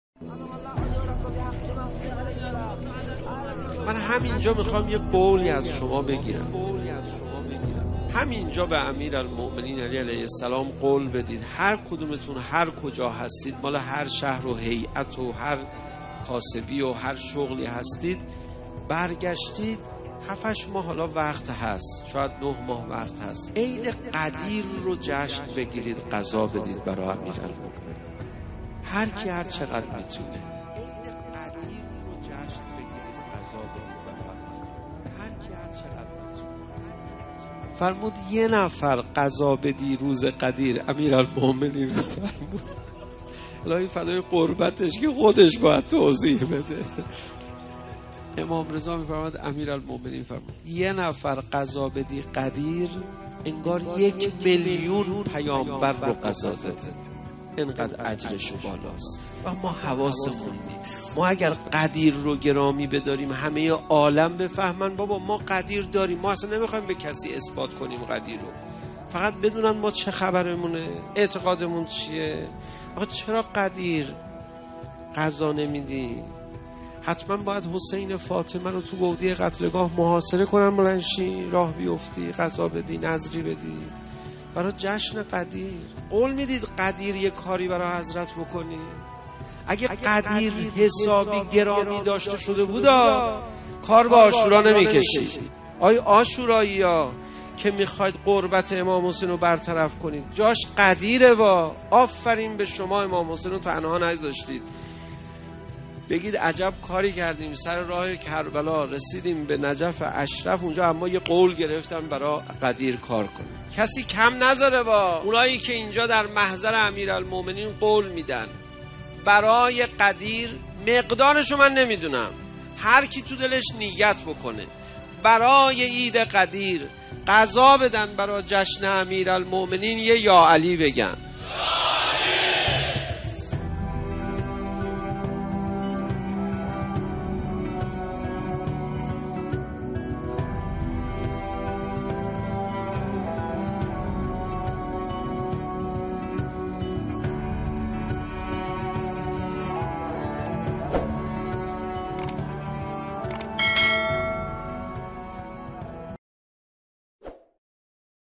Arbaeen vs Ghadir - Agha Ali Raza Panahian Urdu - غدیر
آخر کیوں غدیر کے دن کھانا نہیں دیتے؟ امیر المومنین کو وعدہ کریں آپ جہاں کہیں بھی ہیں عید غدیر پر جشن منائیں، کھانا کھلائیں گے
کیا ضروری ہے کہ فاطمہ زہراسلام اللہ علیہ کا حسینؑ قتلگاہ میں محاصرہ کیا جائےپھر آپ نظر و نیاز کریں؟